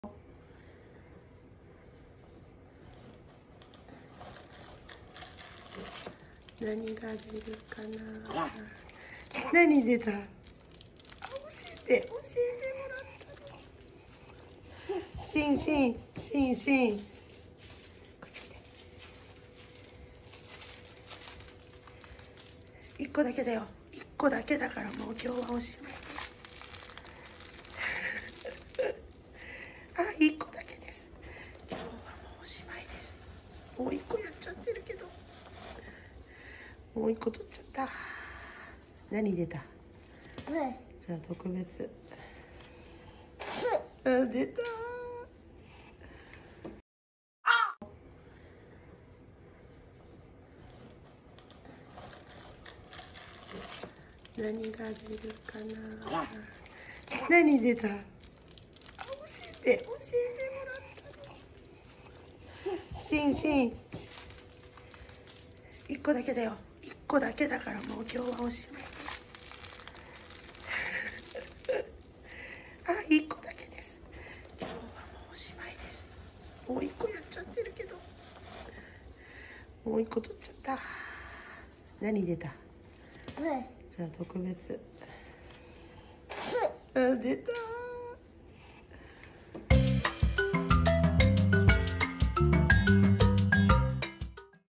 初めてのアドベントカレンダー
初めてのアドベントカレンダー！1日1個だよと言っても２こ、３こ開けてしまう．．．おせんべいが出た時の盛り上がりに注目です！